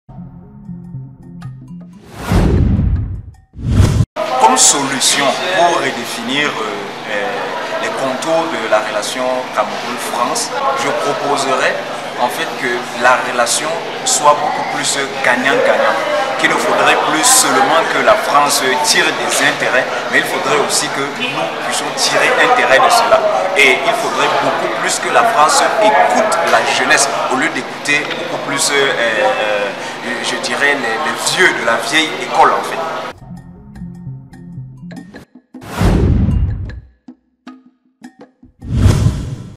HACKATHON: INTERVENANT NGAOUNDERE (2)
EnRouteVersLeSommetAfriqueFrance hackatoncitoyenEn amont du SommetAfriqueFrance qui se tiendra à Montpellier en juillet 2021, un Hackathon citoyen a été organisé samedi, 17 avril 2021 à l’échelle nationale.

 Rassemblant quatre-vingt (80) jeunes à Yaoundé, Douala, Dschang et Ngaoundéré, cet évènement a permis de recueillir les attentes de la jeunesse engagée du Cameroun, et de formuler des propositions fortes, pertinentes et étonnantes pour redéfinir les fondamentaux de la relation entre la France et le continent Africain sur 4 thématiques clés pour le pays et pour la France.
Nous vous proposons de revivre ces moments de reflexion et de débats à Yaoundé, Douala, Dschang et Ngaoundéré.